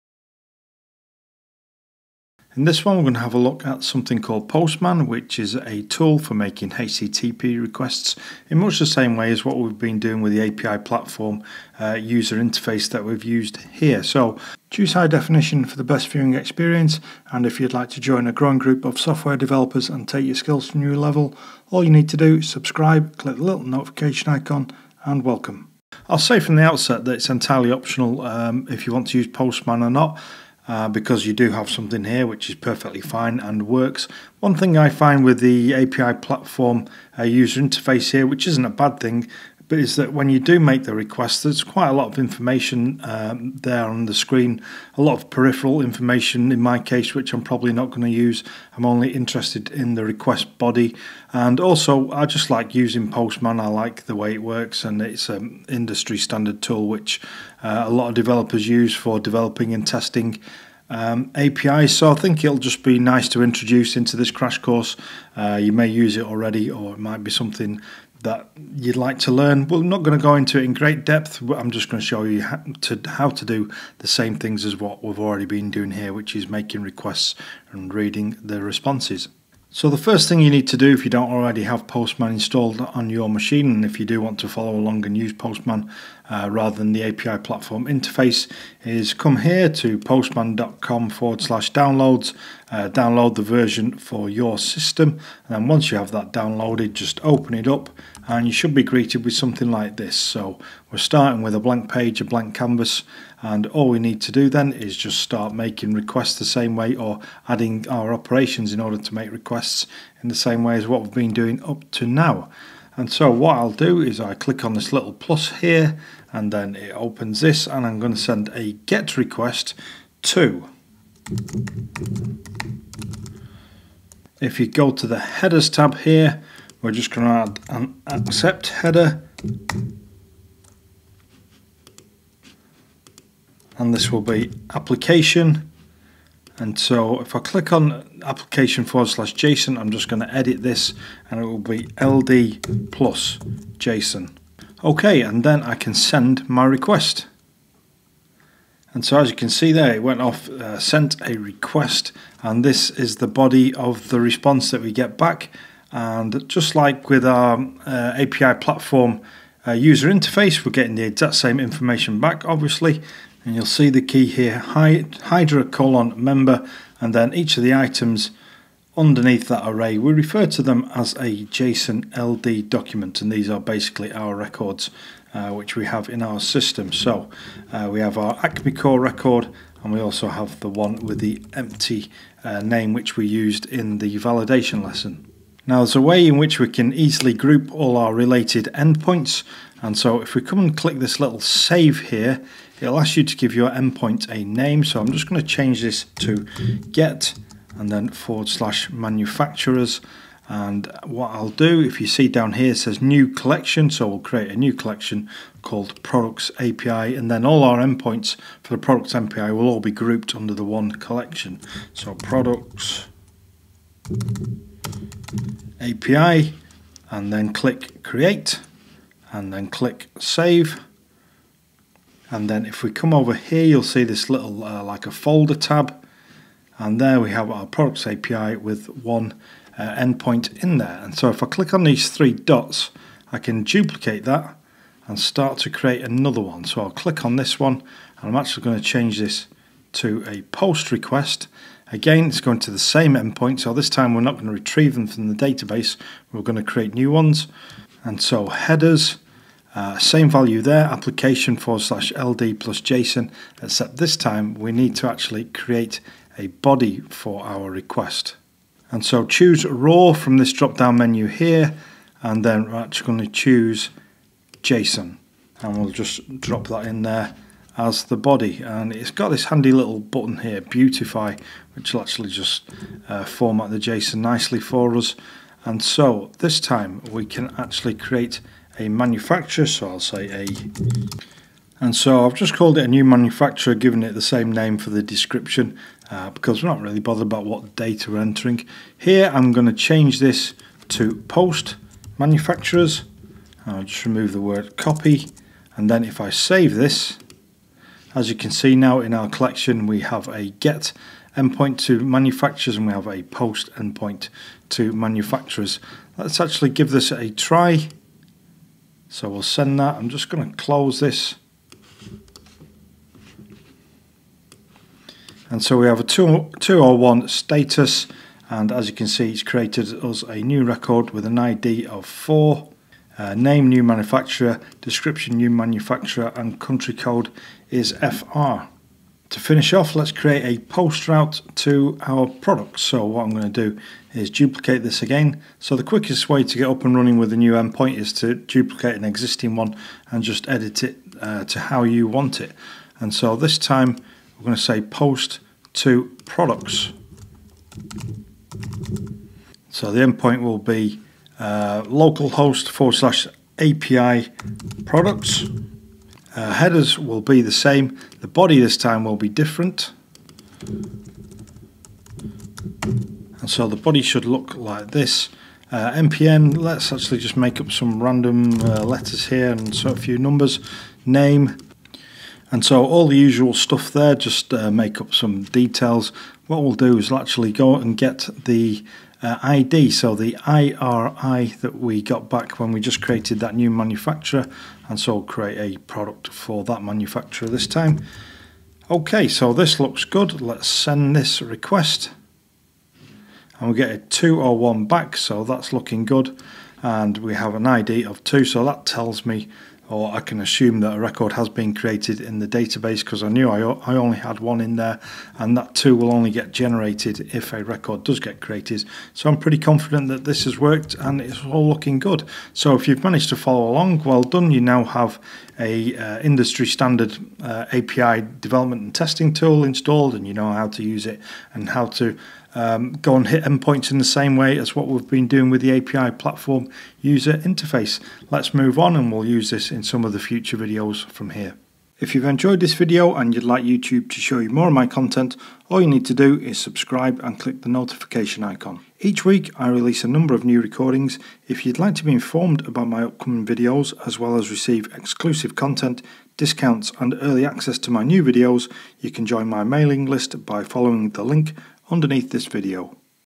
API Platform Crash Course Part 6: Using Postman
ℹ This is a crash course on the API Platform which is intended to get you writing your own API's as quickly as possible. In part 6, I'll show you how to send requests to our API using Postman which is a popular HTTP client tool used by developers.

💻If you are just consuming your own API and you want traditional session authentication then check out this Symfonycasts tutorial: